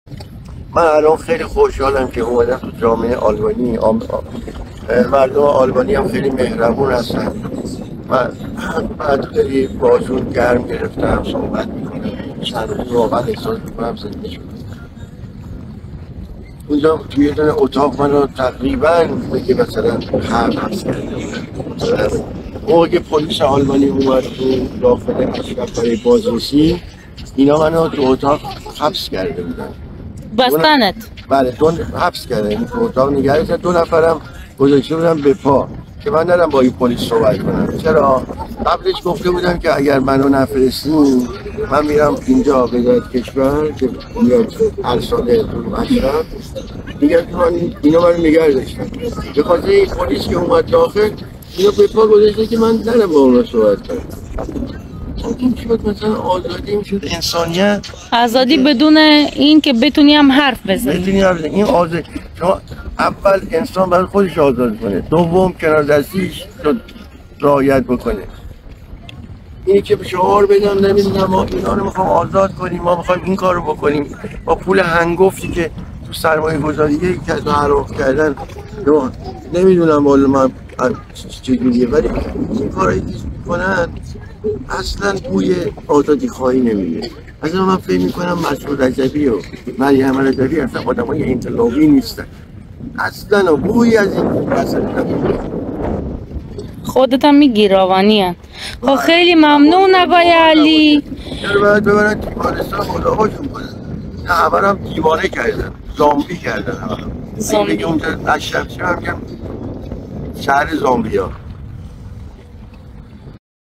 افشاگری علی اصغر زمانی عضو سابق مجاهدین که اخیرا از گروهک جدا شده است
علی اصغر زمانی از اعضای سابق گروهگ منافقین که چند وقت پیش از آن جدا شد، از ماهیت ضد بشری و زندگی بسیار عذاب آور و به دور از آزادی در این گروهک سخن گفته، که حتی در زمان حمله پلیس آلبانی به اشرف ۳ که در آنجا حضور داشته است، او را حبس کرده و اجازه بیرون آمدن به آن نداده اند...